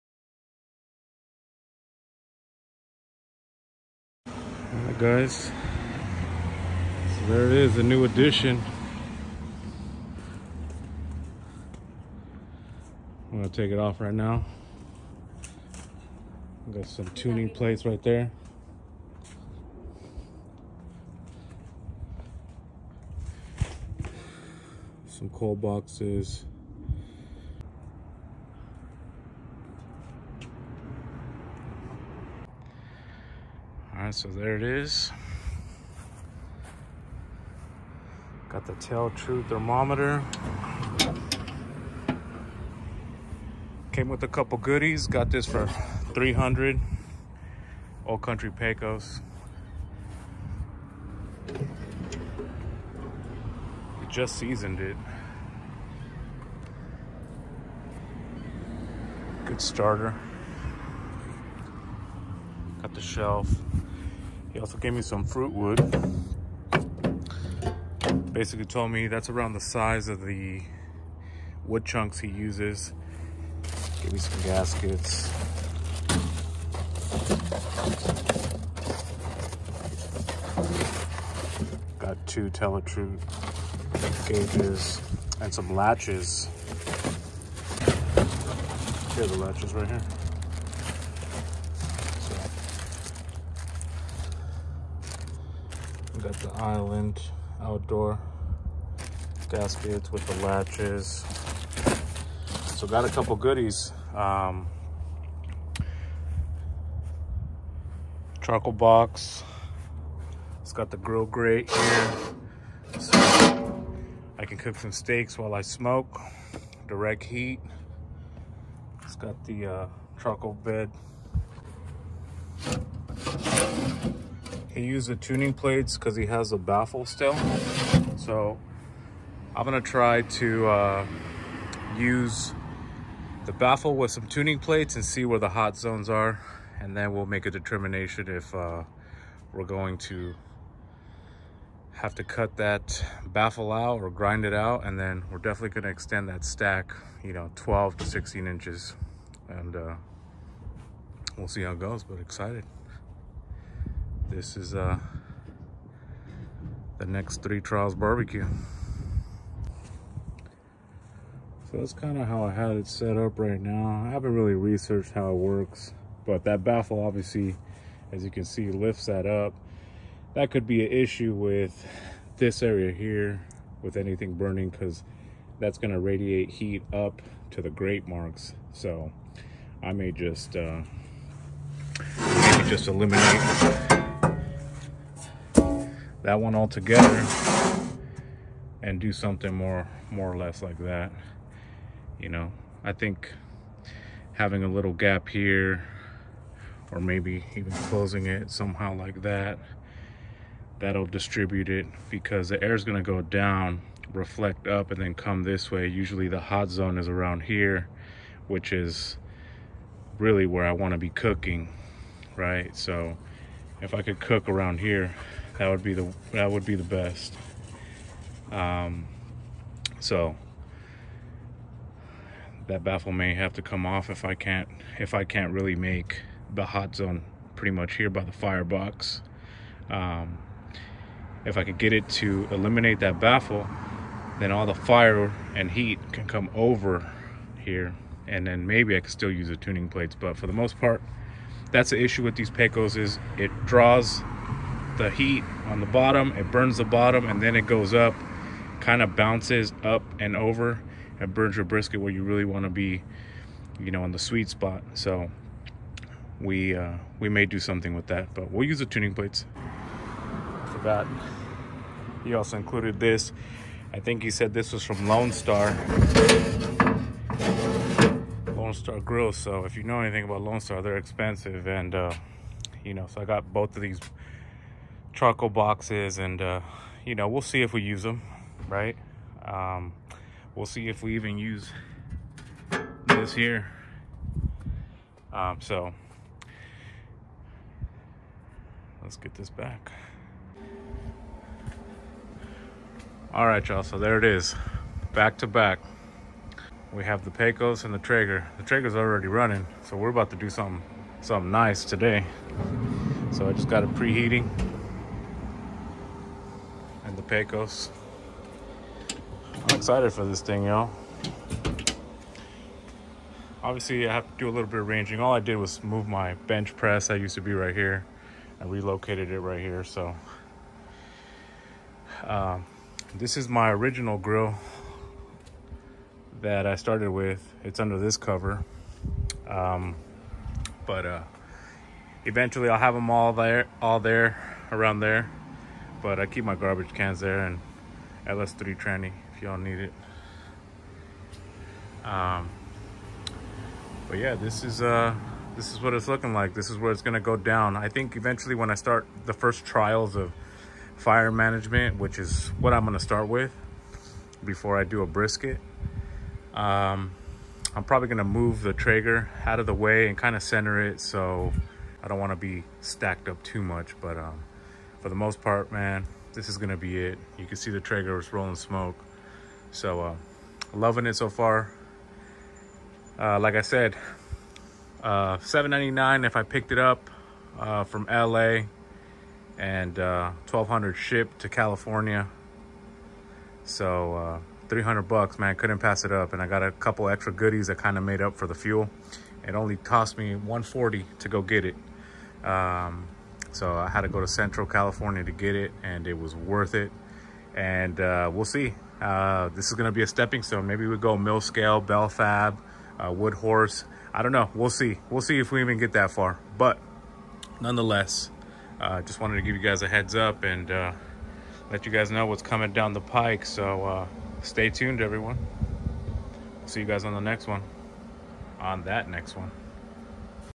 New Smoker : Old Country - Pecos
New Addition- scored on a baby offset smoker with some mods for $300 , Old Country Pecos , came with some tel tru gauges I might re-drill , some lavalock gaskets ,tuning plates and charcoal boxes one from lone star grill . Lots of ideas and cooks coming up but first we have to figure this thing out ,  see if we can get to know the pit and learn to manage the fire along with the cold and hot spots to determine if that baffle will stay or not.